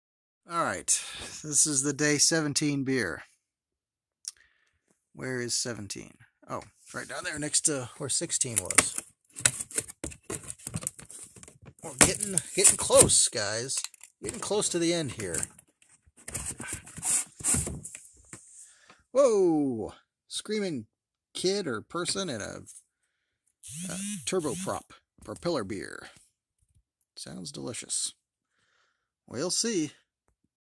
Brewer's Advent Calendar 2021 Day 17Intro
Beer #17 in the Costco Brewer's Advent Calendar!